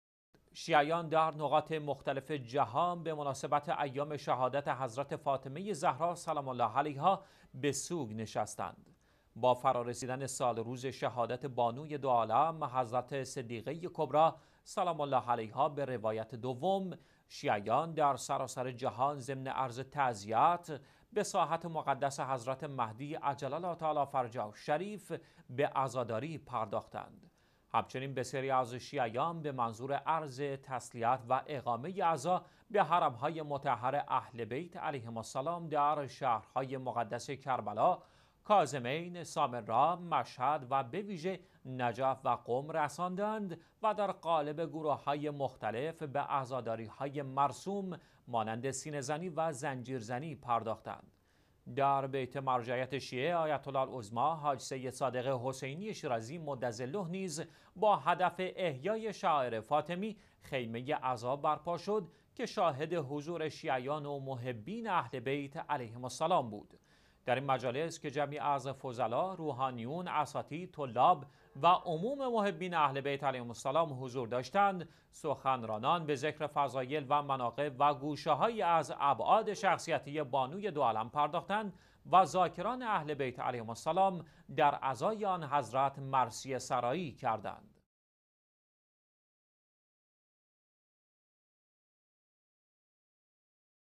جهان در عزای سالروز شهادت صدیقه کبری حضرت فاطمه زهرا علیها السلام
شیعیان در نقاط مختلف جهان، در سالروز شهادت «صدیقه کبری فاطمه_زهرا سلام الله علیها» به عزاداری پرداختند.
با فرا رسیدن سالروز شهادت بانوی دو سرا حضرت صدیقه کبری سلام الله علیها (به روایات دوم) شیعیان در سراسر جهان ضمن عرض تعزيت به ساحت مقدس حضرت_مهدی عجل الله تعالی فرجه الشریف، به عزاداری پرداختند.
همچنین تعداد زیادی از شیعیان، خود را برای عرض تسلیت به حرم های مطهر اهل بیت علیهم السلام در شهر های مقدس کربلا، کاظمین، سامرا، مشهد و به خصوص نجف و قم رسانیده و در قالب گروه های مختلف به عزاداری های مرسوم مانند سینه زنی و زنجیر زنی پرداختند.
بیت مرجعیت شیعه «آیت الله العظمی سید صادق شیرازی»مدظله، نیز با برپایی خیمه عزا با هدف احیای شعائر فاطمی، شاهد حضور شیعیان و محبین اهل_بیت علیهم السلام بود.
در این مجالس که با حضور جمعی از فضلاء، روحانیون، اساتید، طلاب و عموم محبان اهل بیت علیهم السلام برگزار گردید، سخنرانان به ذکر فضایل و مناقب و گوشه هایی از ابعاد شخصیتی بانوی دو سرا پرداخته و ذاکران اهل بیت علیهم السلام در عزای آن حضرت مرثیه سرایی کردند.